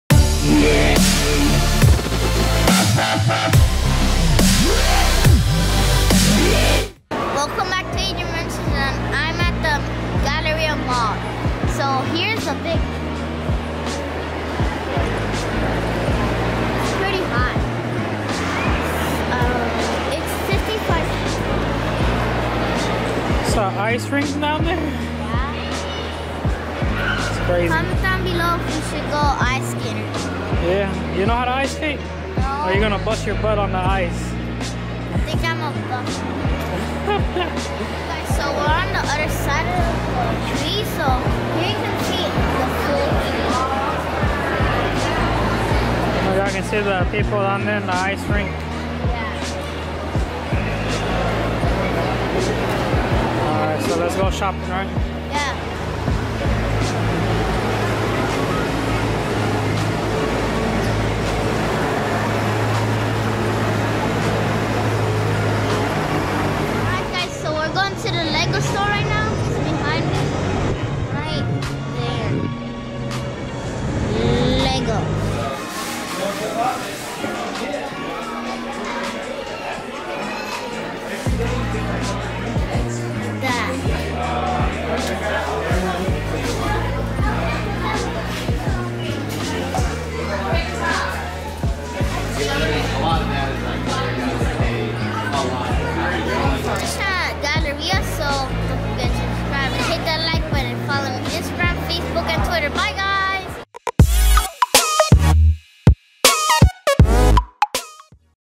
HUGE 55 Foot Christmas Tree Houston Galleria. 2018
Was around the galleria area and decided to come check out the awesome huge tree they put up for the upcoming holidays. Like always thanks for all the support really appreciate it!